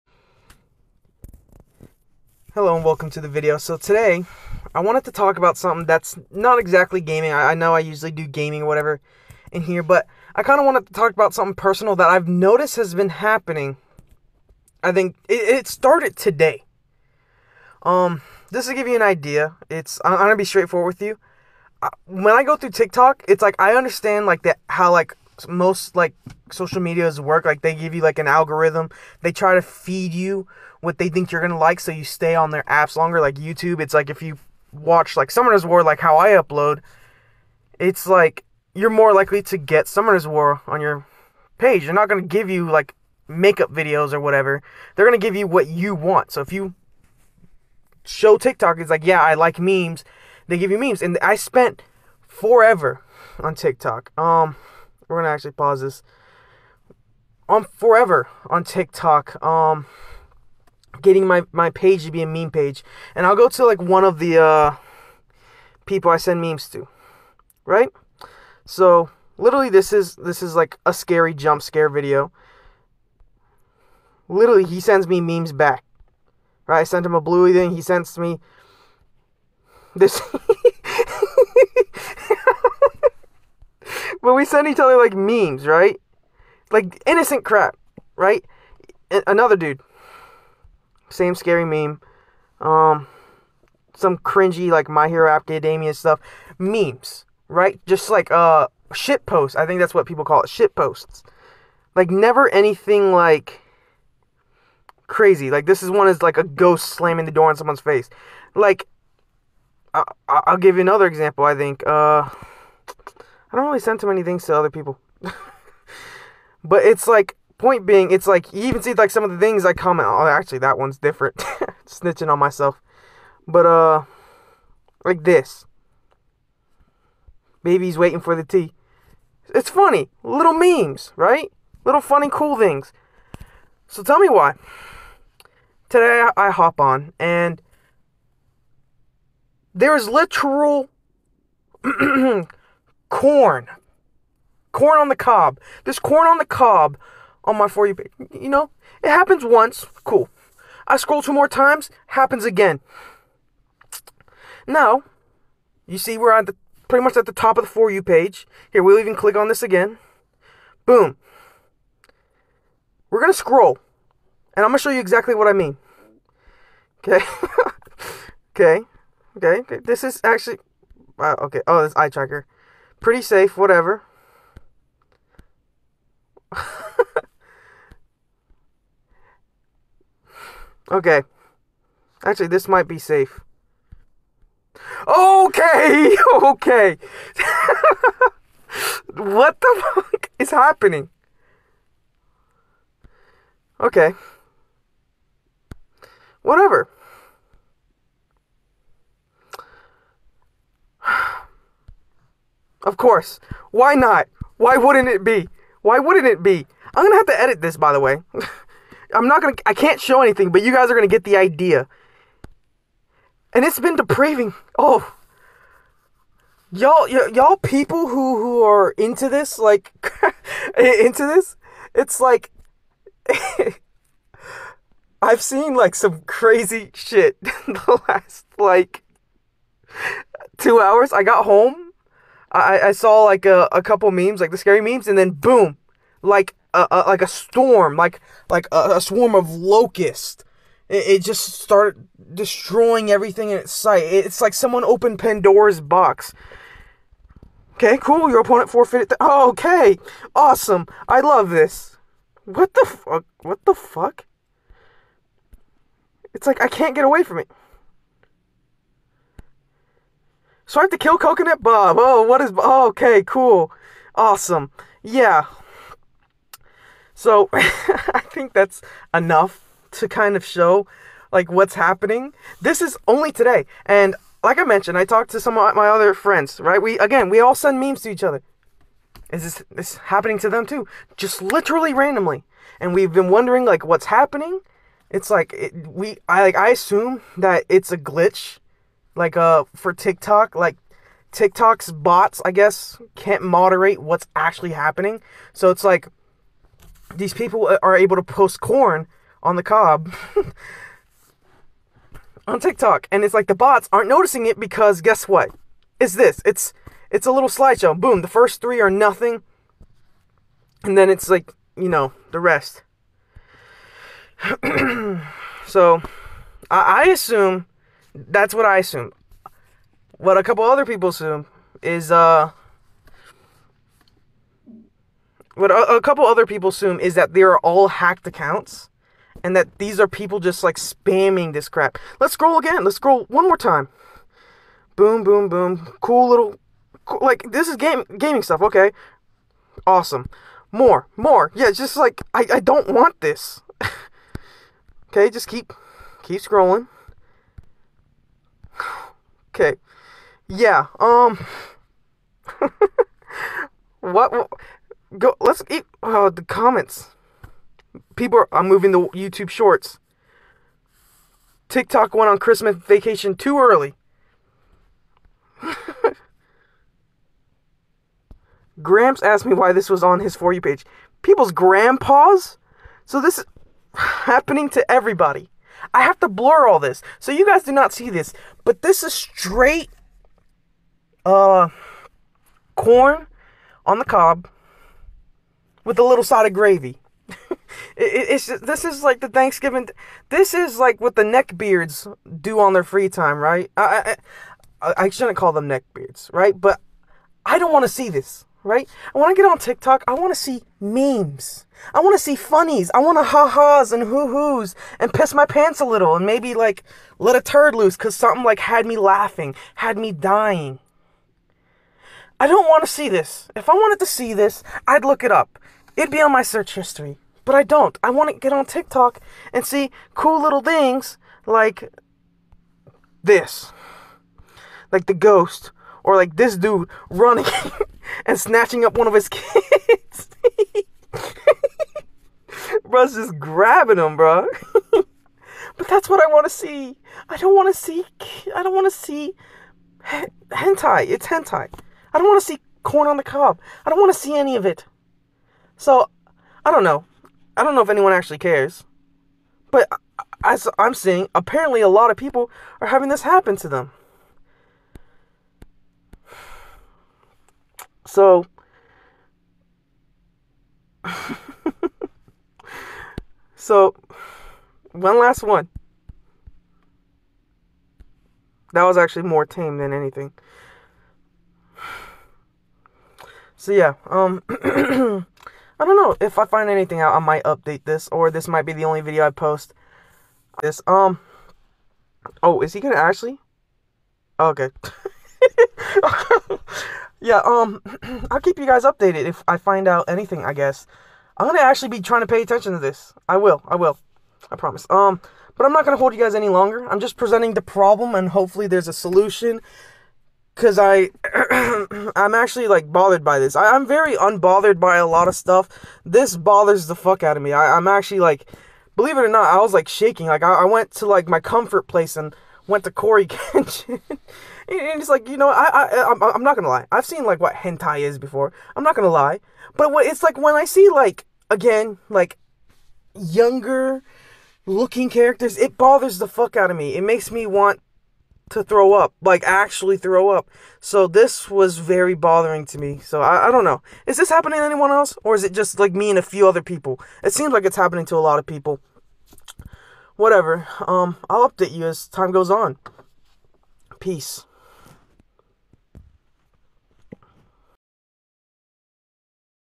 TikTok has officially given up
Before anyone says it either I have specifically for the last year or two went out of my way to block tik thots or whatever to have memes only so the idea that it's my "search history" or whatever causing it to show up just isn't correct.